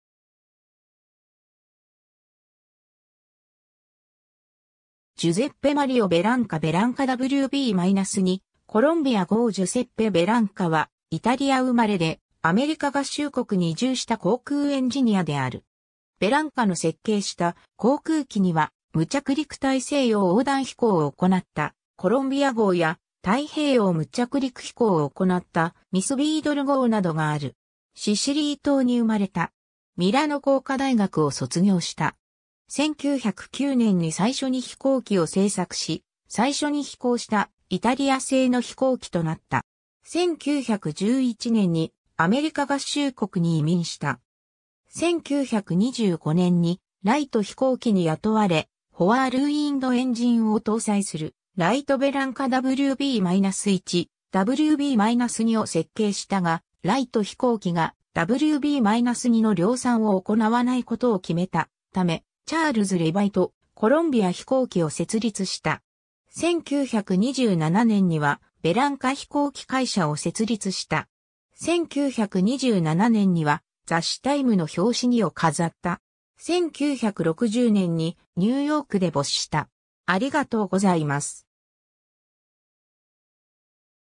ジュセッペ・ベランカ
Giuseppe Mario Bellanca (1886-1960) ベランカ WB-2 「コロンビア」号 （1927年） ジュセッペ・ベランカ（Giuseppe Bellanca、1886年3月19日 - 1960年12月26日）はイタリア生まれで、アメリカ合衆国に移住した航空エンジニアである。
ベランカの設計した航空機には、無着陸大西洋横断飛行を行った、コロンビア号や、太平洋無着陸飛行を行った、ミス・ビードル号などがある。
 シシリー島に生まれた。
ミラノ工科大学を卒業した。
1909年に最初に飛行機を製作し、最初に飛行したイタリア製の飛行機となった。
1911年にアメリカ合衆国に移民した。
1925年にライト飛行機に雇われ、ホワールウィンドエンジンを搭載するライト・ベランカ WB-1、WB-2を設計したが、ライト飛行機がWB-2の量産を行わないことを決めたため、チャールズ・レヴァイと、コロンビア飛行機を設立した。
1927年にはベランカ飛行機会社（Bellanca Aircraft Company）を設立した。
1927年には雑誌タイムの表紙にを飾った。
1960年にニューヨークで没した。